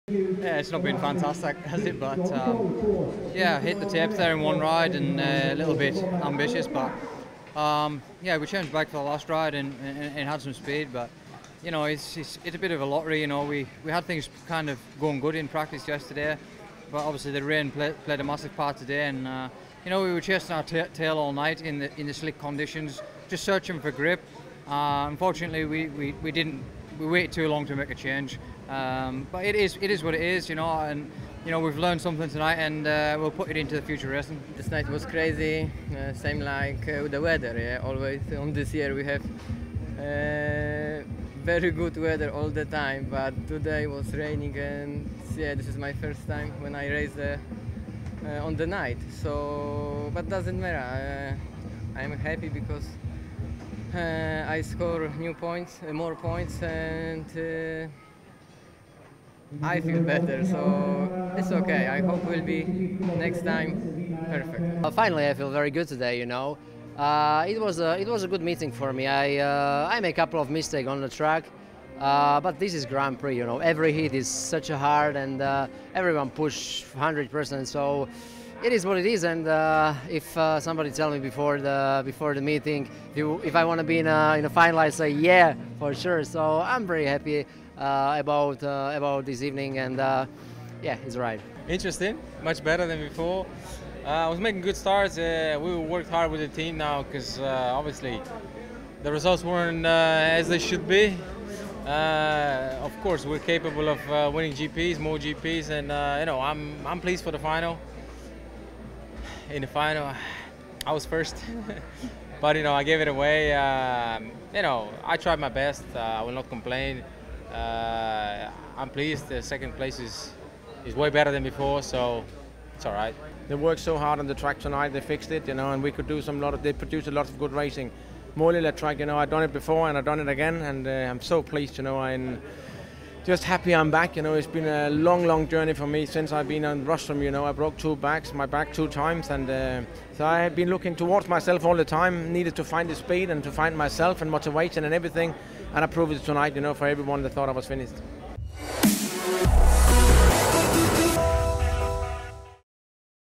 Betard Scandinavian SGP: Rider Reaction
Hear what the FIM Speedway Grand Prix stars had to say after tonight's Betard Scandinavian SGP in Malilla ...